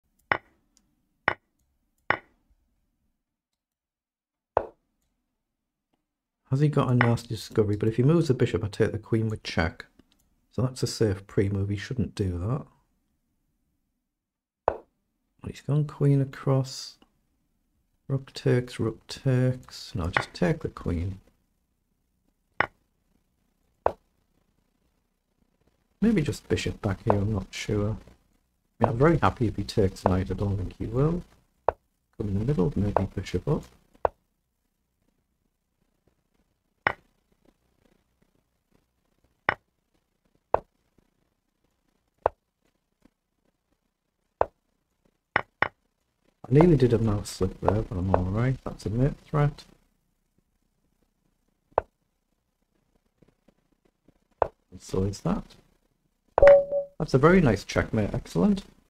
paulw7uk chess v 1778 rooks combine for nice side checkmate lichess.org
paulw7uk chess. this is 1-min bullet chess on lichess.org.
when we join the game we are just about to go up material and then a case of trying to simplify the position and we manage to end with a nice checkmate using the power of the 2 rooks.